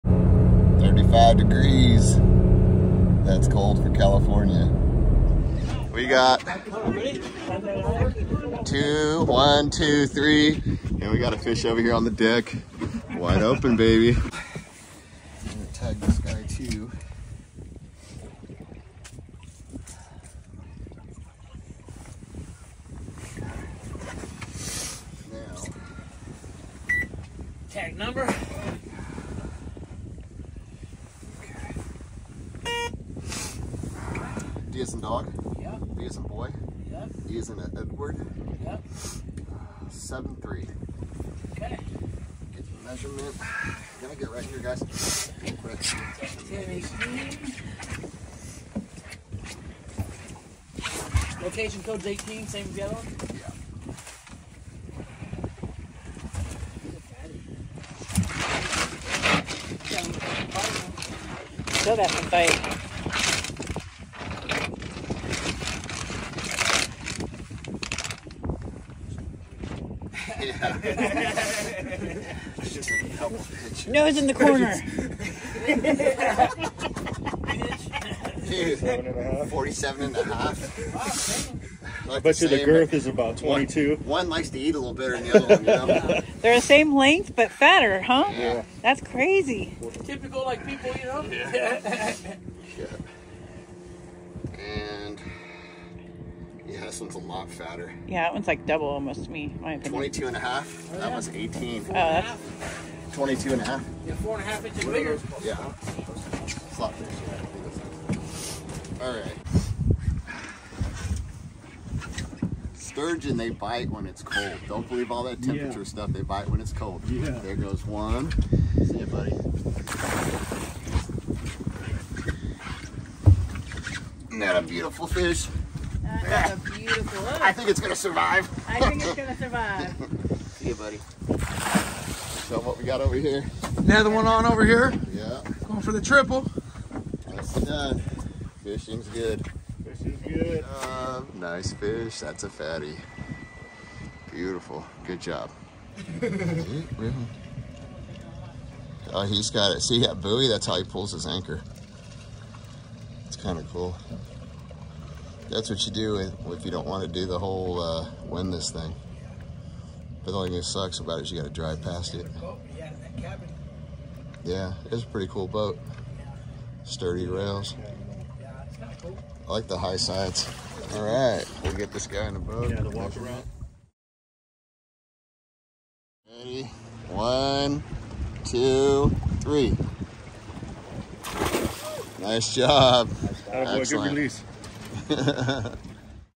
San Francisco Bay | Sturgeon Fishing Charter | California Delta
Gatecrasher Fishing is currently scanning, tagging and releasing Sturgeon in the California Delta. This project is designed to assist our state in forming future population estimates.